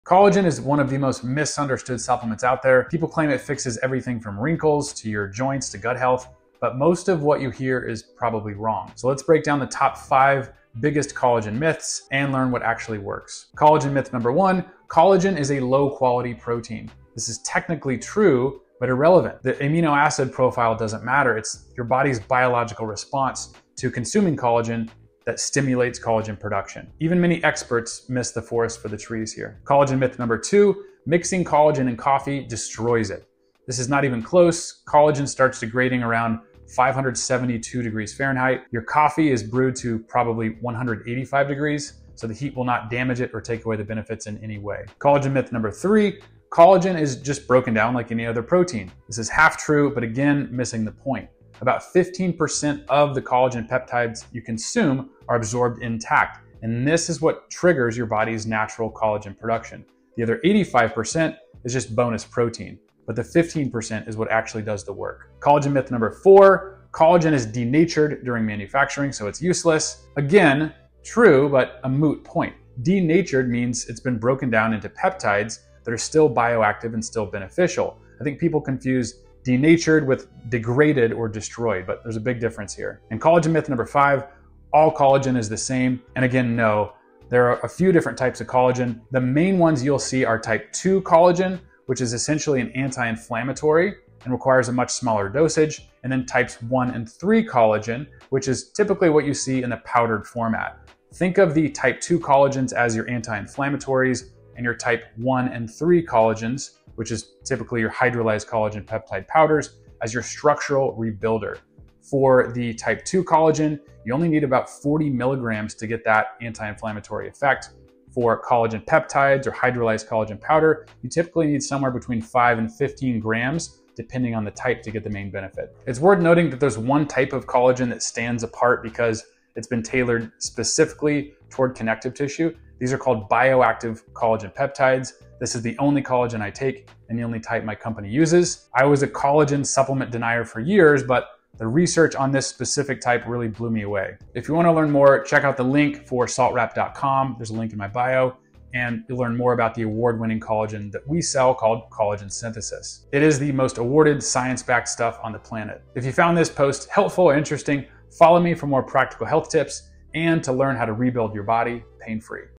Top 5 Collagen Myths
Collagen is one of the most misunderstood supplements out there.

People claim it fixes everything from wrinkles to your joints to gut health.

But most of what you hear is probably wrong.

Let's break down the top five biggest collagen myths and learn what actually works.

Collagen myth number one: Collagen is a low quality protein.

This is technically true, but irrelevant.

The amino acid profile doesn't matter.

It's your body's biological response to consuming collagen that stimulates collagen production.

Even many experts miss the forest for the trees here.

Collagen myth number two: Mixing collagen and coffee destroys it.

This is not even close.

Collagen starts degrading around 572 degrees Fahrenheit.

Your coffee is brewed to probably 185 degrees, so the heat will not damage it.

Collagen myth number three: Collagen is just broken down like any other protein.

This is half true, but again missing the point.

About 15% of the collagen peptides you consume are absorbed intact and this triggers your body's natural collagen production.

The other 85% is just bonus protein, but the 15% does the work.

Collagen myth number four: Collagen is denatured during manufacturing, so it's useless.

Again, true, but a moot point.

Denatured means it's broken down into peptides that are still bioactive and beneficial.

People confuse denatured with degraded or destroyed.

There's a big difference here.

Collagen myth number five: All collagen is the same.

No, there are different types.

Type 2 collagen is essentially an anti-inflammatory and requires a much smaller dosage.

Types 1 and 3 collagen is what you see in powdered format.

Think of type 2 as your anti-inflammatory and types 1 and 3 as your structural rebuilders.

For type 2, you only need about 40 milligrams.

For collagen peptides or hydrolyzed collagen powder, you need somewhere between 5 and 15 grams.

There's one type that stands apart: bioactive collagen peptides.

These are tailored specifically toward connective tissue.

This is the only collagen I take and the only type my company uses.

I was a collagen supplement denier for years, but the research on this specific type really blew me away.